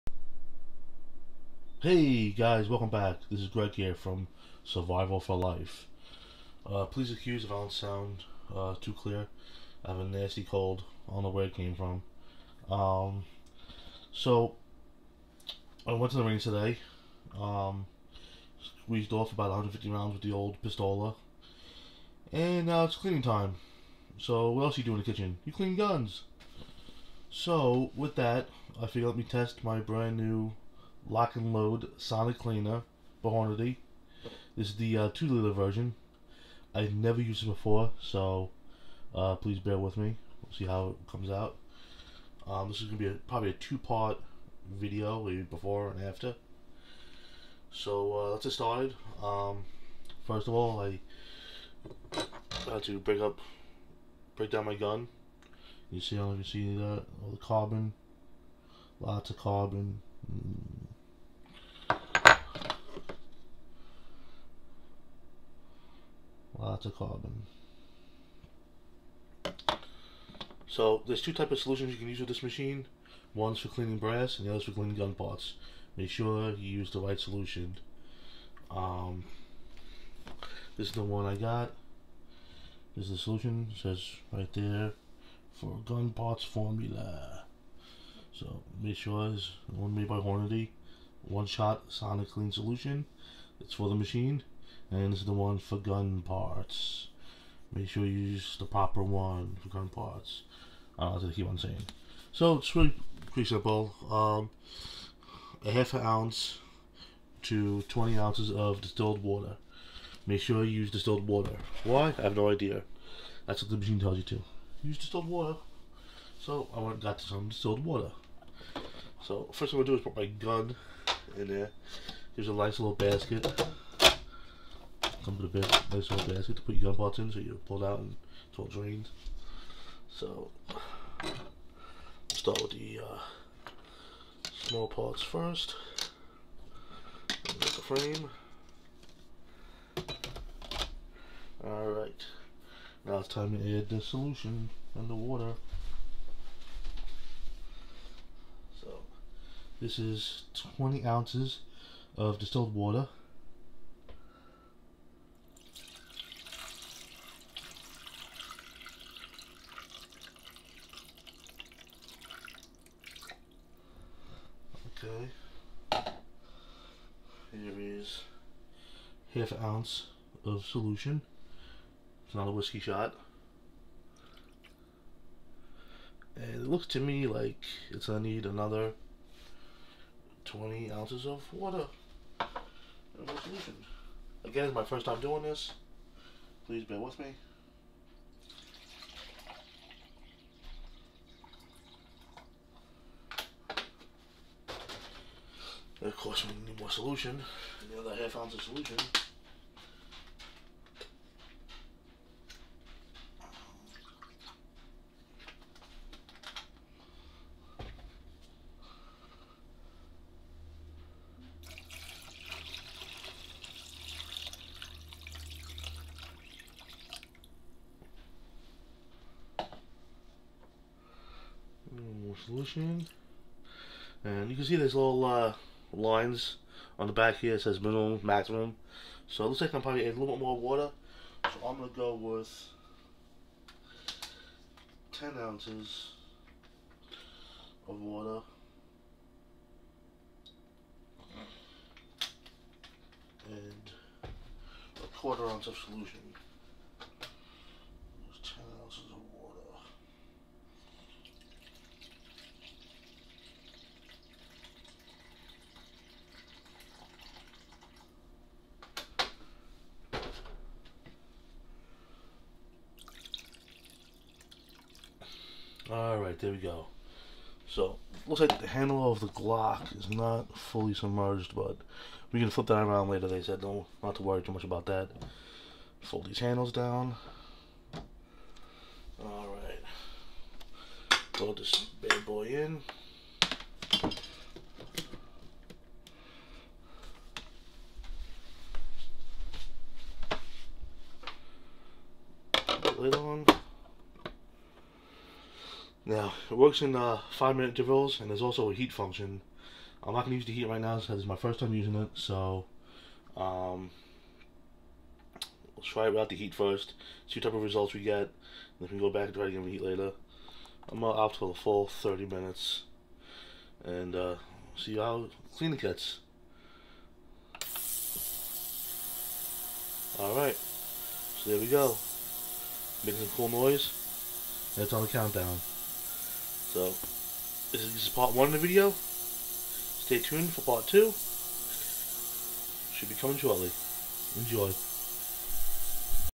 Lock and load sonic cleaner test run
Testing out my new sonic cleaner by hornady the lock and load 2L part 1